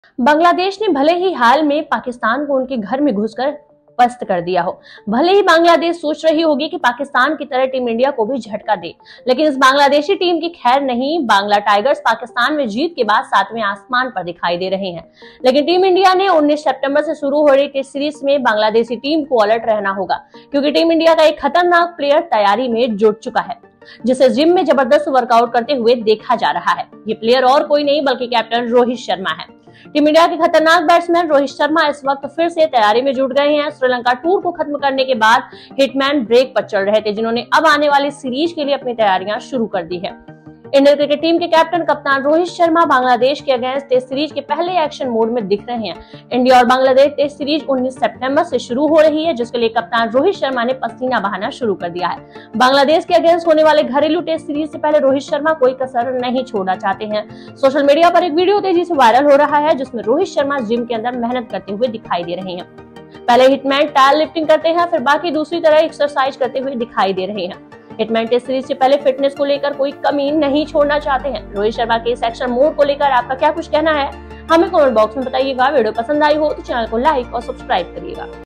Ind vs Ban test squad update 2024 | Rohit Sharma prepares for the Bangladesh tour 2024
Rohit Sharma, one of the most elegant batsmen of modern-day cricket and a brilliant captain for the Indian cricket team, is now gearing up for his next big challenge. After leading India to significant victories, Rohit’s focus has shifted to the upcoming Test series against Bangladesh, set to begin on September 19.

Known for his stylish batting and calm leadership, Rohit Sharma has consistently proven his mettle on the international stage. Recently, he was spotted in the gym, working on his fitness as part of his preparation for the series. A video of Rohit sweating it out has since gone viral on social media, with fans applauding his dedication and determination.

This Test series against Bangladesh is a crucial step for Team India, and Rohit Sharma’s role as a captain will again be under the spotlight. With his commitment to fitness and strategy, fans eagerly wait to see him back in action, leading the team from the front.